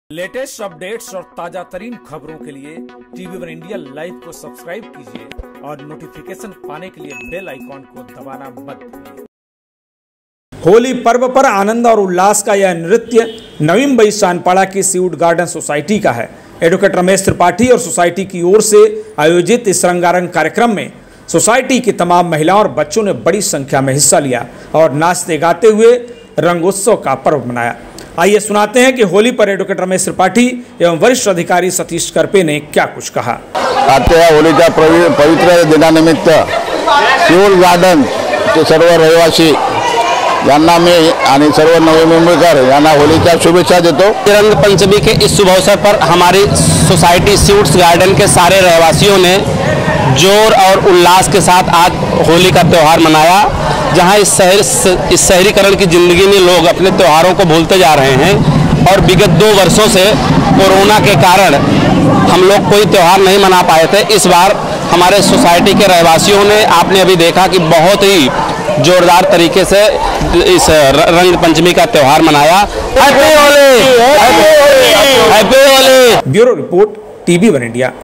Holi2022: Adv. Ramesh Tripathi | सीवुड गार्डन सोसायटी में होली का आनंद उत्सव
होली पर आनंद और उल्लास का यह नृत्य नवी मुंबई सानपाड़ा की सीवुड गार्डन सोसायटी का है. एड. रमेश त्रिपाठी और सोसायटी की ओर से आज यहां रंगारंग कार्यक्रम चला जिसमें सोसायटी की महिलाओं और बच्चों ने भी बड़ी संख्या में हिस्सा लिया और नाचते गाते हुए रंगोत्सव मनाया